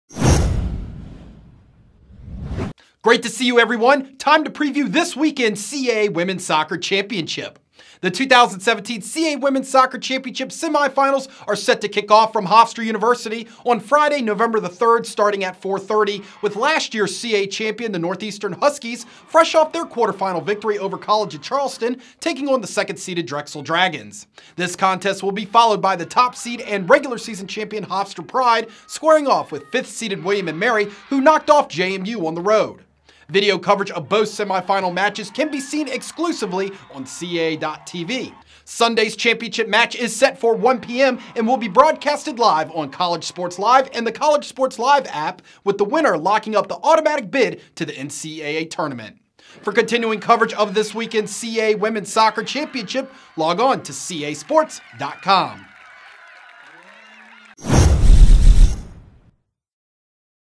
2017 CAA Women's Soccer Championship Preview
The 2017 CAA Women’s Soccer Championship continues on Friday, November 3 with semifinal play at Hofstra Soccer Stadium. Second-seeded Drexel takes on third-seed Northeastern at 4:30 p.m. followed by top-seeded Hofstra going up against fifth-seeded William & Mary at 7:00 p.m.